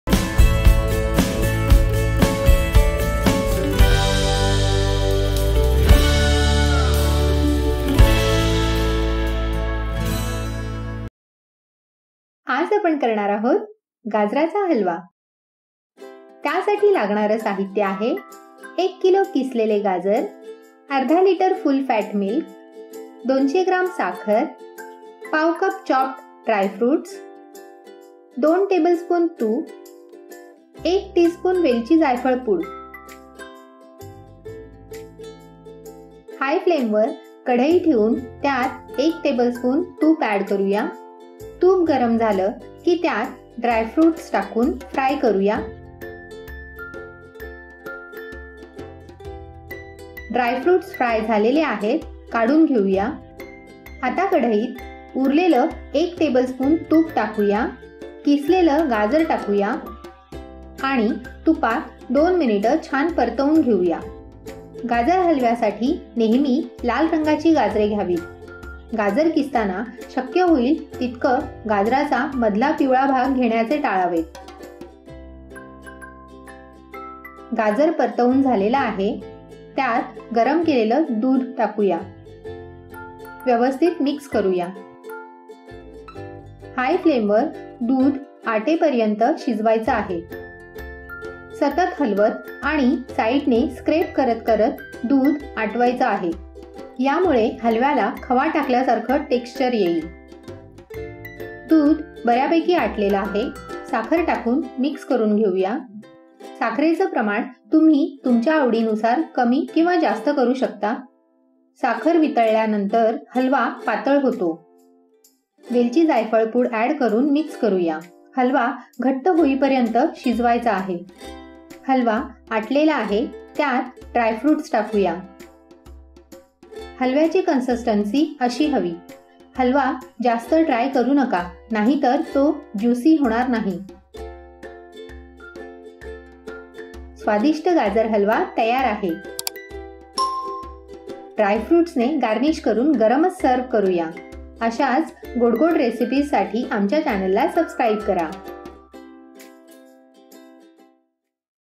Gajaracha Halwa Recipe | How to Make Gajar Halwa | Gajar Halwa Recipe (Easy) | गाजराचा हलवा
About this video:  Gajar ka halwa, also known as Gajorer Halua, Gajrela, Gajar Pak, and Carrot Halwa is a carrot-based sweet dessert pudding. For cooking Gajar ka halwa, a cooker or Kadai is usually preferred. At the festival time, many people prefer Gajar halwa as desserts in their Thali. Gajar ka halwa is a combination of nuts, milk, sugar, khoya, and ghee with grated carrot. In this video, I will show you how to make Gajar Halwa without khoya. Without khoya also, this carrot halwa super delicious and look like it has added khoya into it.